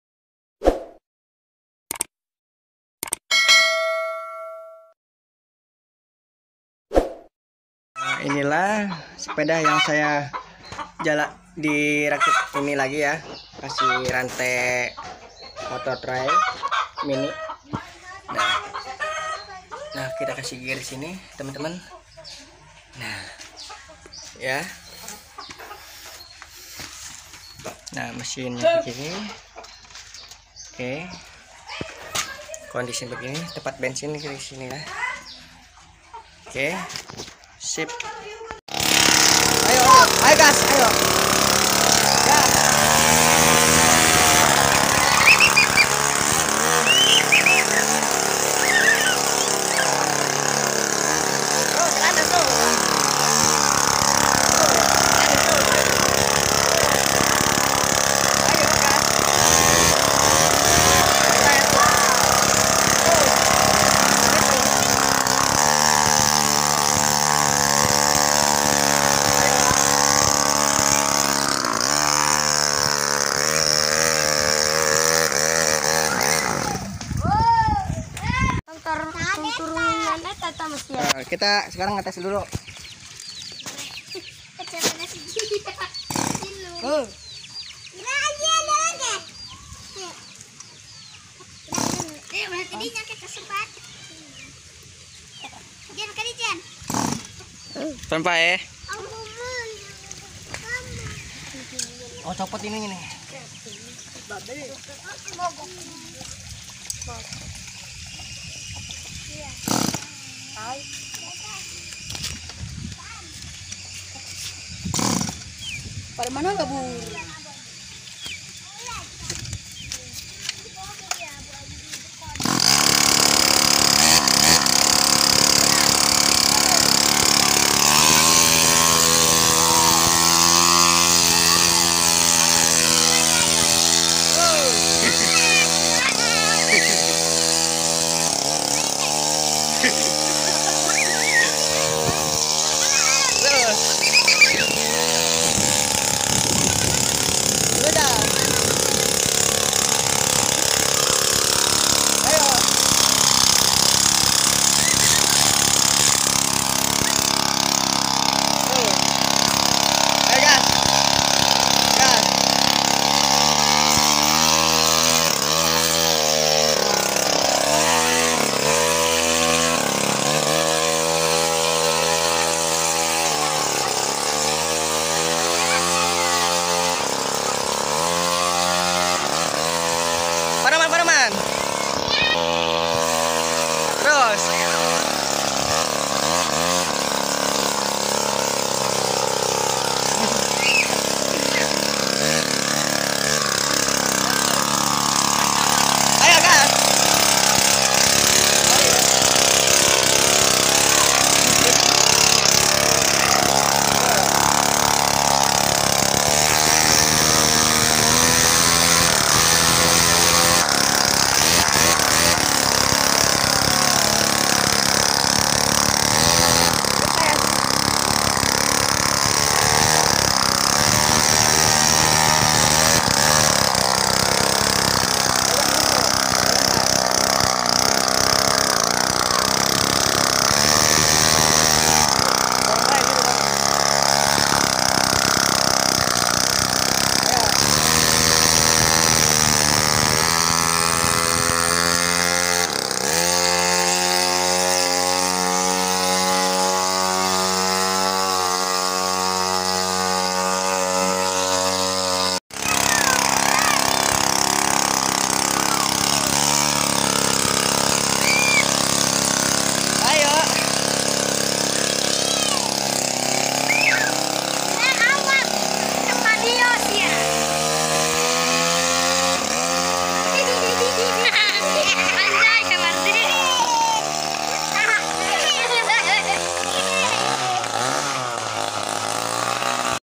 uji coba ngetes motor rakitan sendiri dari mesin potong rumput
motor mesin potong rumput yang paling mantap